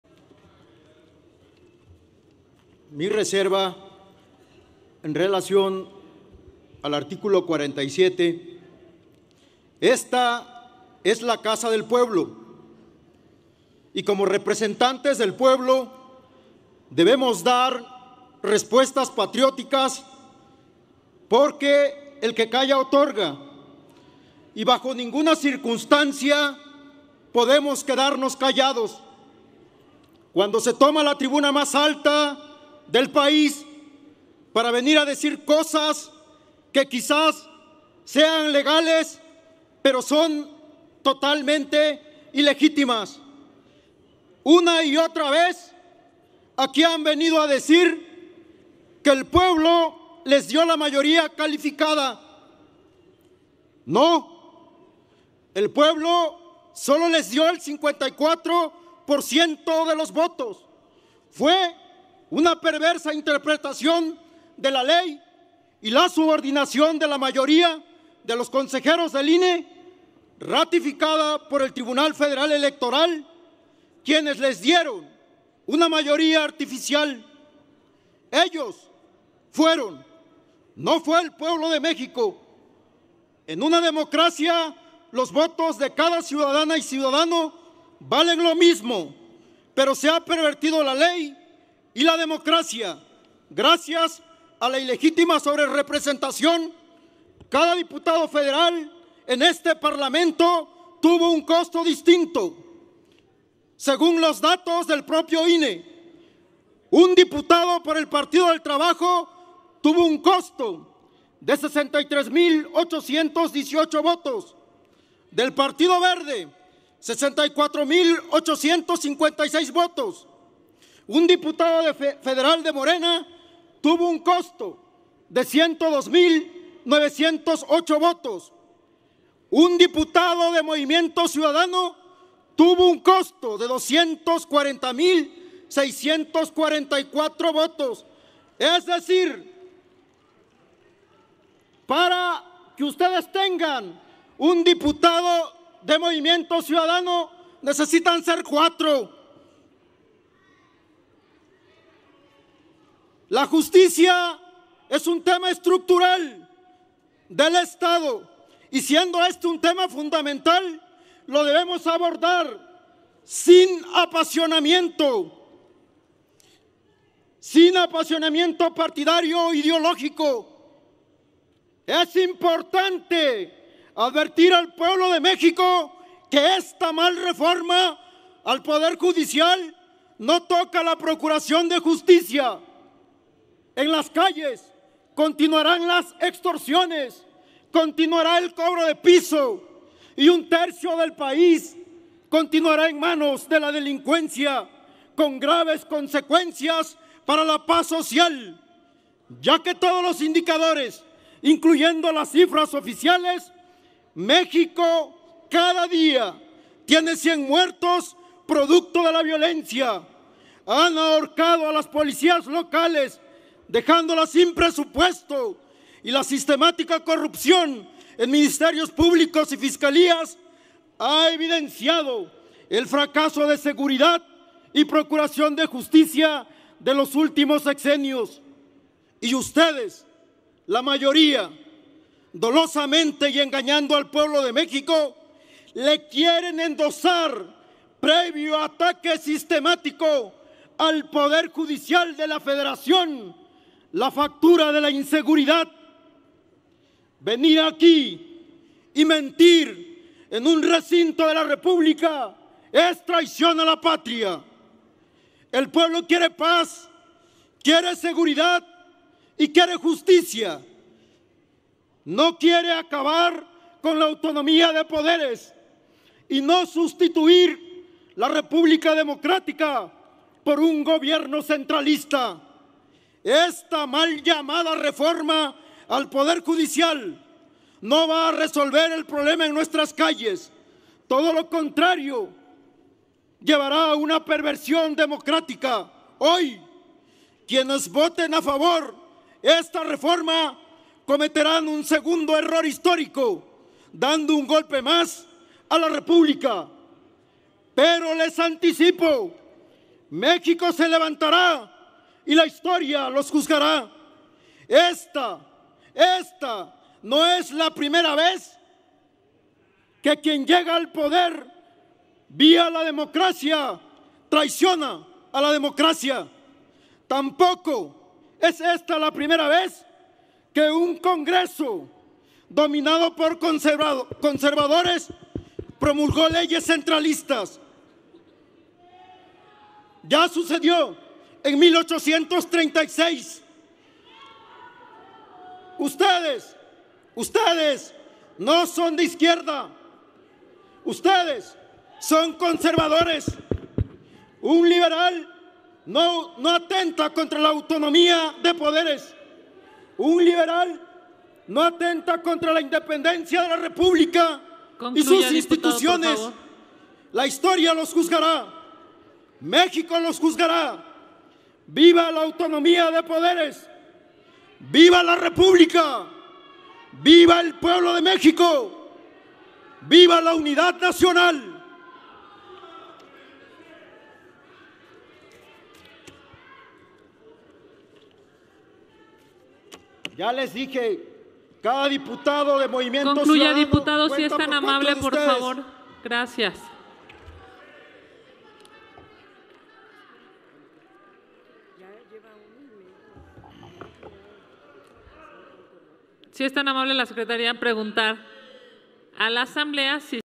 Leyes Reglamentarias de la Reforma al Poder Judicial | Gildardo Pérez Gabino
Como representantes ciudadanos, no podemos quedarnos callados cuando México pasa por una crisis de seguridad e impartición de justicia. Nuestro país merece paz y justicia, con un sistema que salvaguarde a las y los ciudadanos.

Escucha a Gildardo Pérez Gabino